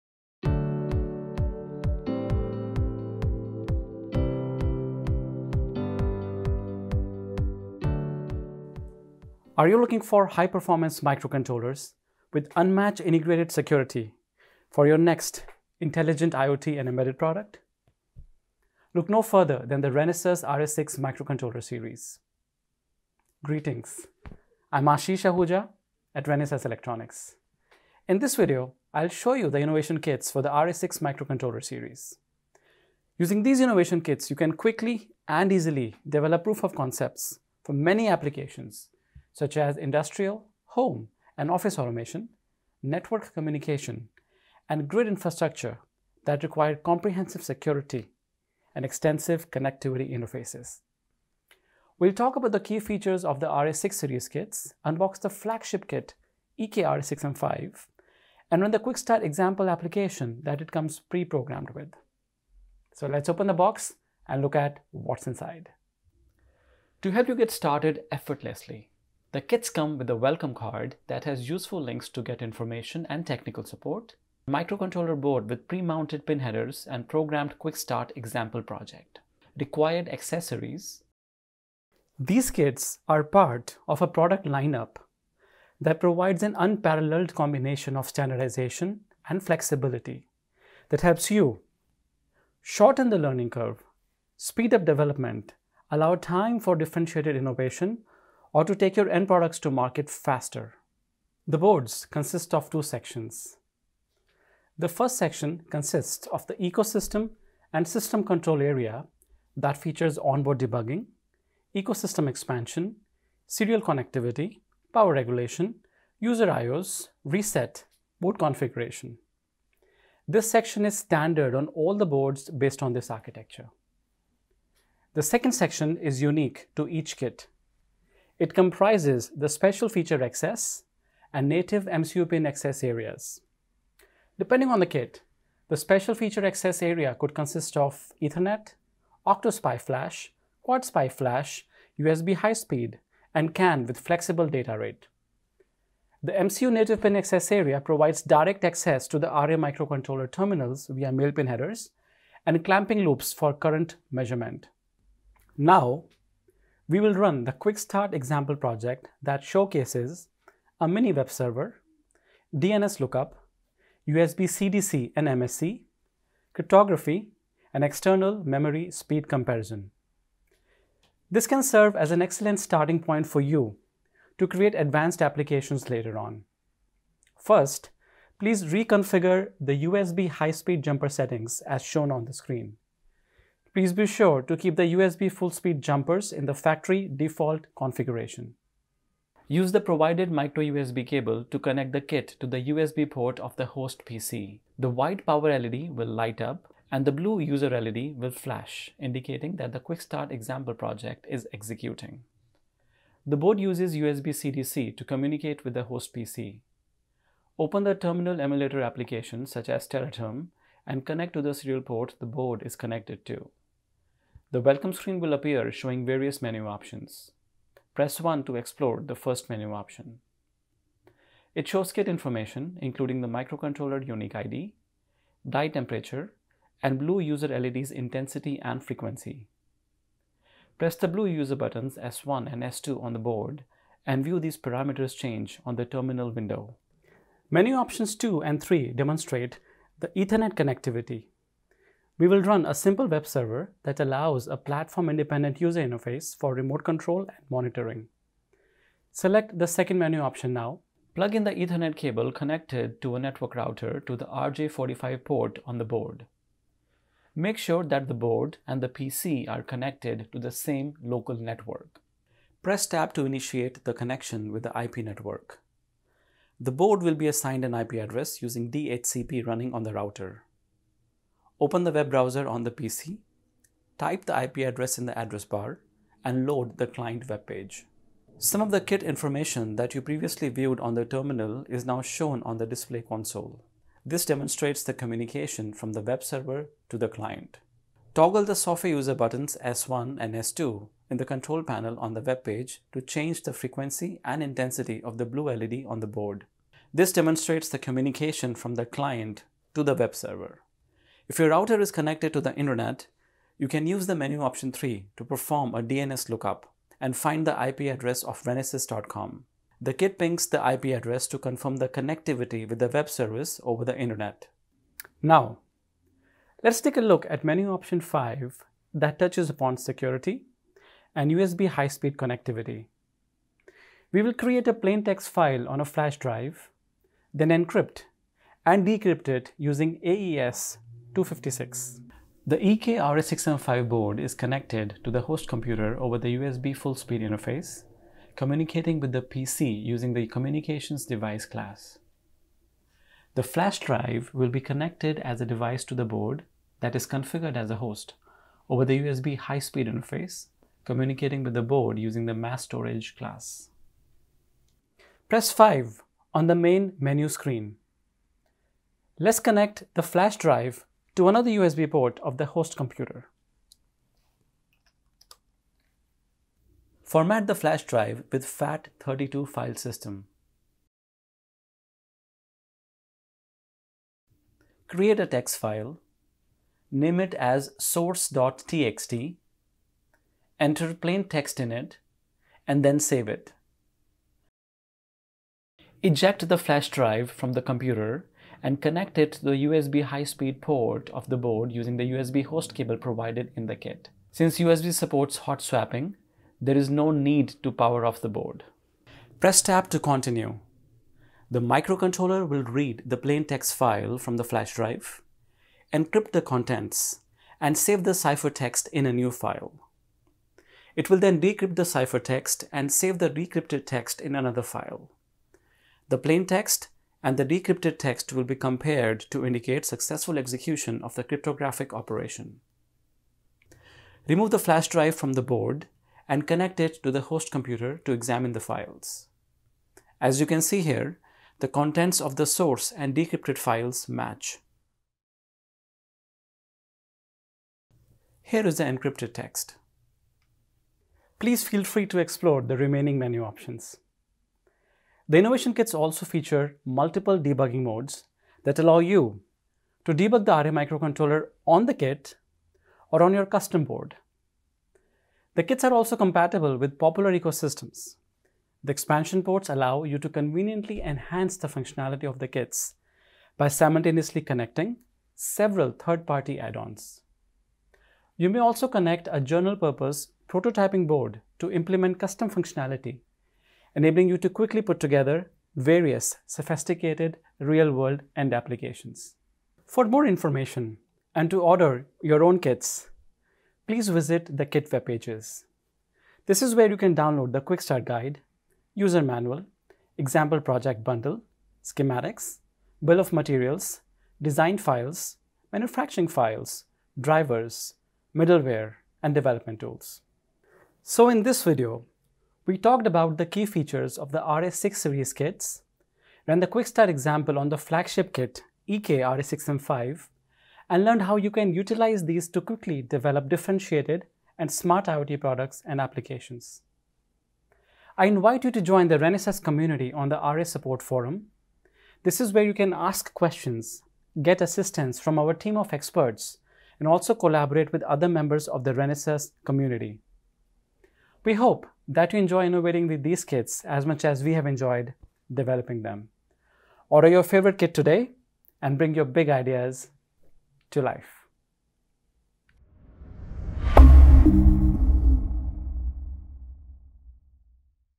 RA6 Series Kits for Renesas Advanced (RA) Microcontrollers
Kickstart IoT and embedded systems development using Renesas EK-RA6M5, EK-RA6M4, and EK-RA6M3 Evaluation Kits for RA6 MCU Series.